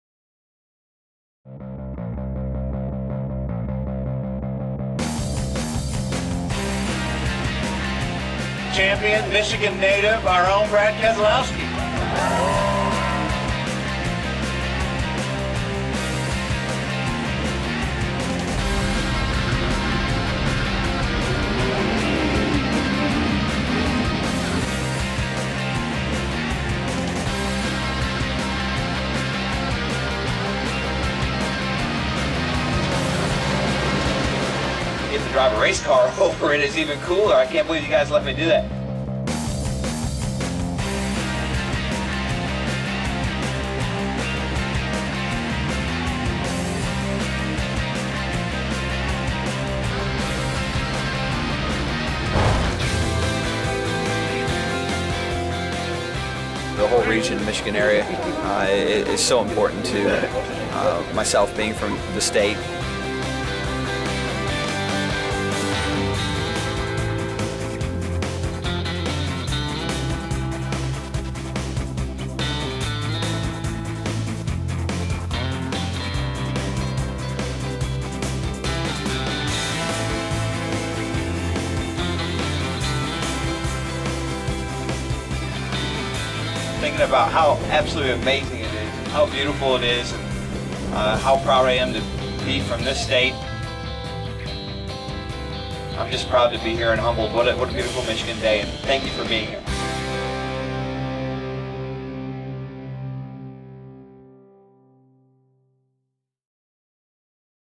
Brad Keselowski Crosses the Mackinac Bridge
NASCAR Sprint Cup Champion Brad Keselowski crosses the Mackinac Bridge in his No. 2 Miller Lite Ford Fusion.